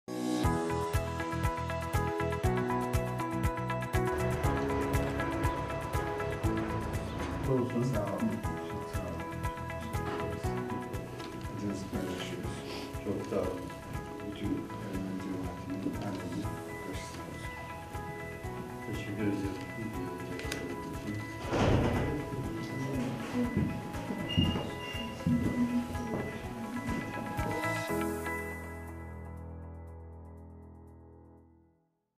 Kumkapı Türkiye Ermenileri Derneği Taziye Ziyareti
Meclis Üyesi Adayımız Selina Doğan ile birlikte Türkiye Ermenileri Patriği Mesrob Mutafyan’ın vefatı dolayısıyla Kumkapı Ermeni Patrikhanesi’ne taziye ziyaretinde bulunduk. Görüşmenin ardından Mutafyan için açılan taziye defterini imzaladık. Kendisine bir kez daha Allah’tan rahmet diliyoruz, ışıklar içinde uyusun.